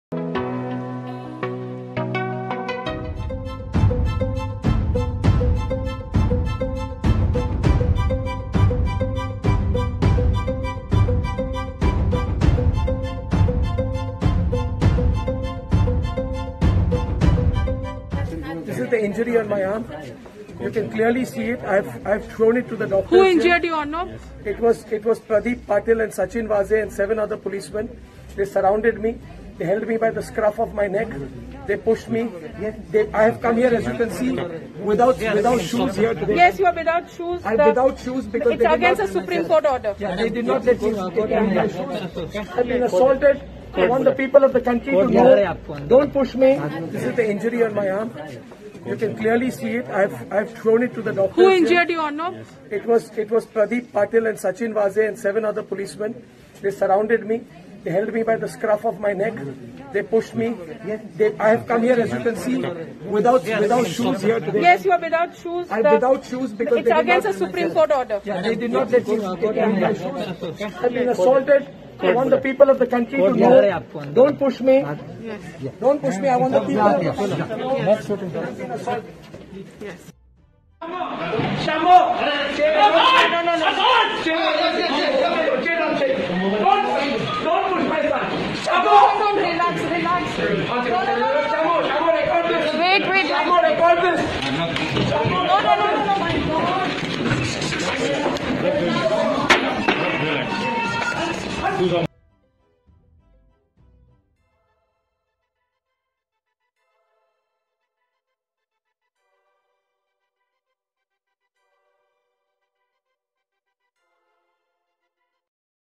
Arnab Goswami को 14 दिनों की Judicial Custody में भेजा गया, 18 नवंबर तक जेल में रहेंगे
रिपब्लिक टीवी के एडिटर-इन-चीफ अर्नब गोस्वामी को 14 दिनों की न्यायिक हिरासत में भेजा गया। रायगढ़ के अलीबाग कोर्ट ने अर्नब को 14 दिनों की न्यायिक हिरासत में भेजा। अर्नब गोस्वामी 18 नवंबर तक न्यायिक हिरासत में रहेंगे।